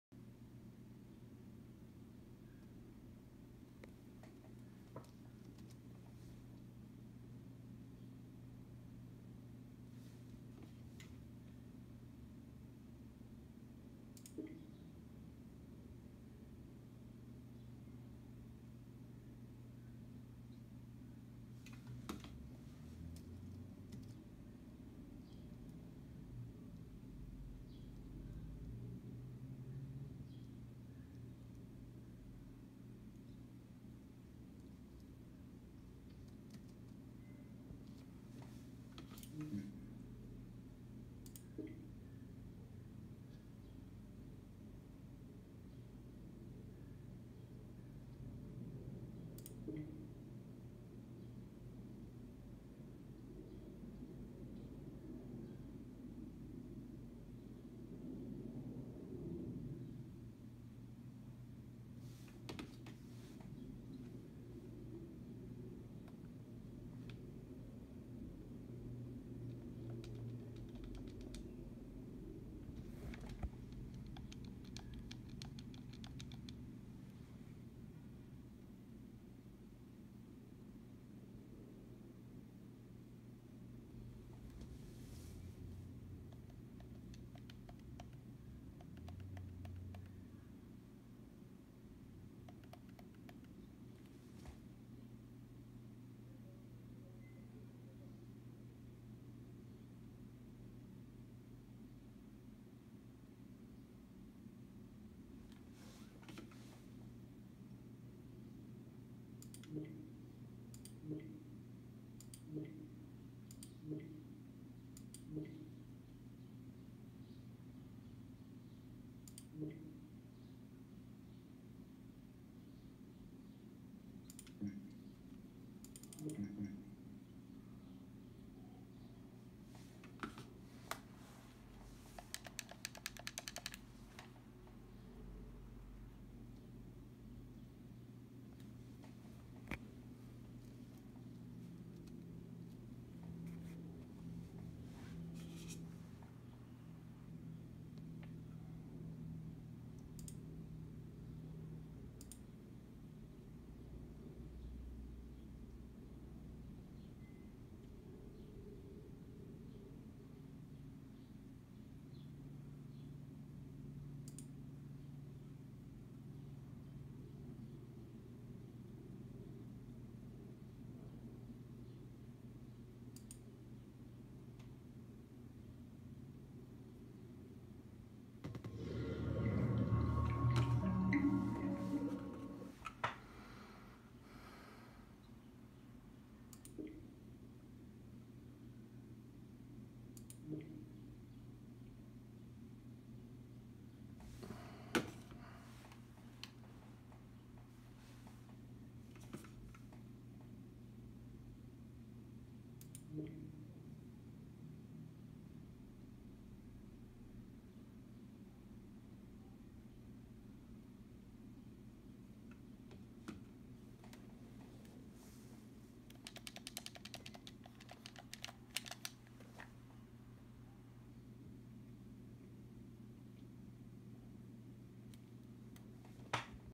Come on Steam!
Mac Steam not allowing PS4 controller connected by Bluetooth.

Come on YouTube! You seriously reduced the quality to 240p? I can't even read it now wtf.

Oh and that point at 0:43 where it looks like it's fixed when I turn off PS4 Config Support is bs, I went through everything you see on the video like 4 times before I recorded it and not once did it stop going in and out like that when I did that before. It slowed down when I turned it off but didn't stop like that for so long before recording it. But it wasn't working anyway.